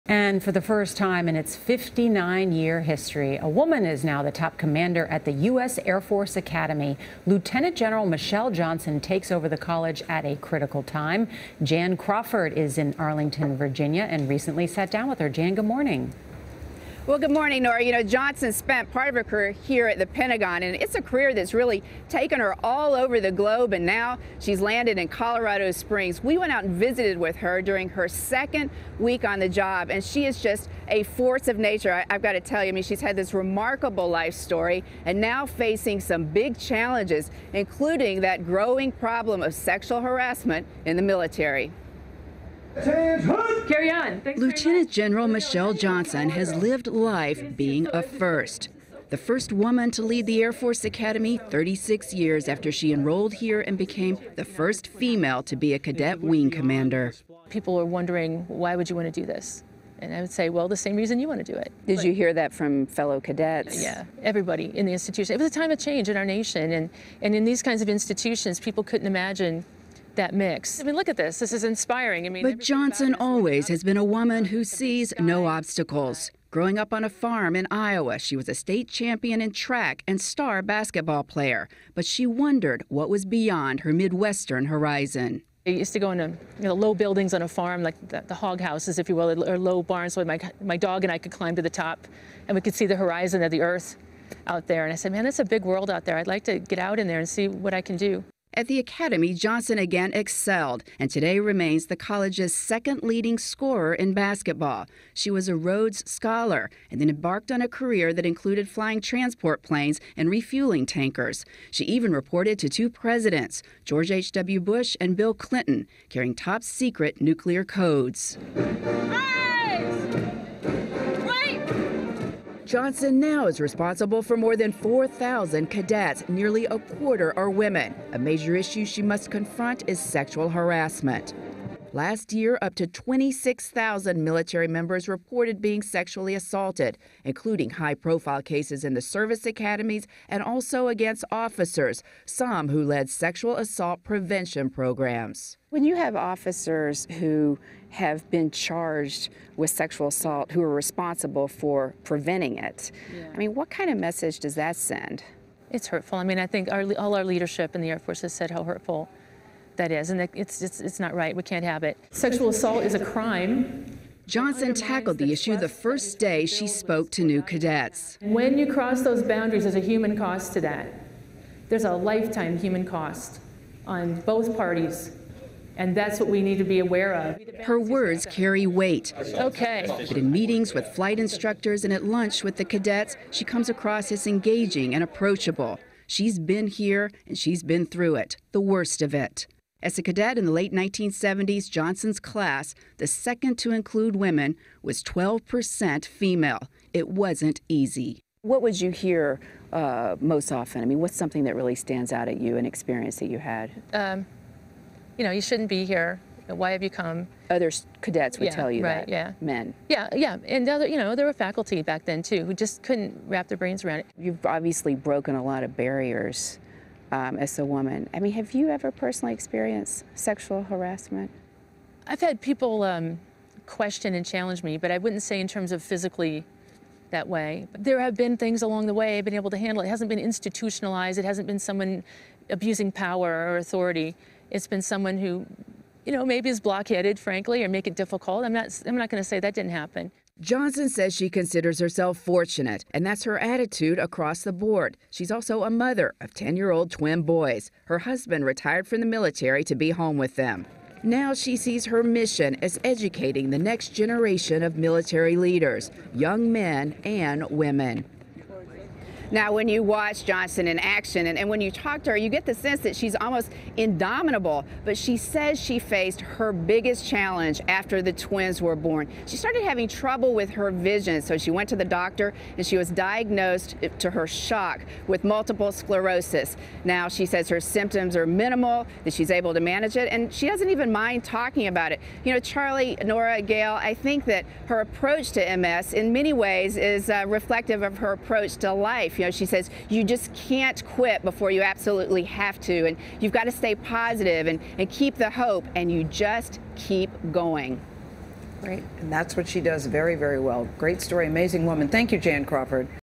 First female air force academy leader takes over
For the first time in its 59-year history, a woman is now the top commander at the U.S. Air Force Academy. Jan Crawford talks to Lt. Gen. Michelle Johnson.